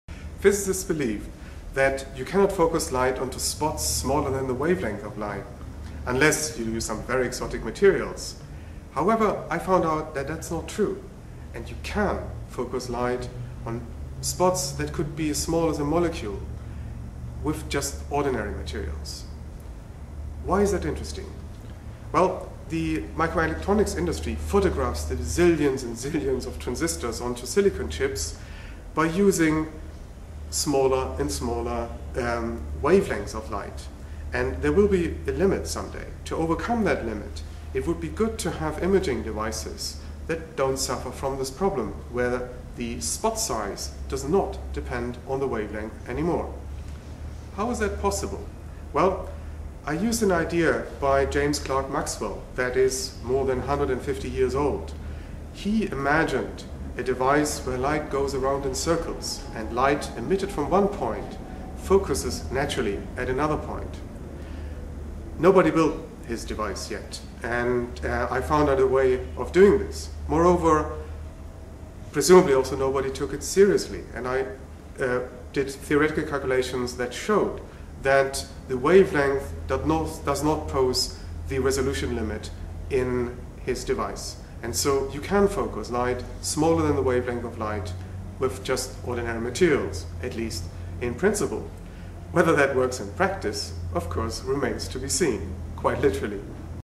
Ulf Leonhardt: Perfect image without metamaterials. and a reprieve for silicon chips
Since 2000, Sir John Pendrys work on metamaterials has been at the vanguard of efforts to create a perfect image  images with perfect resolution that can stem from light being moved in odd directions to create, among other tricks of the light, the illusion of invisibility.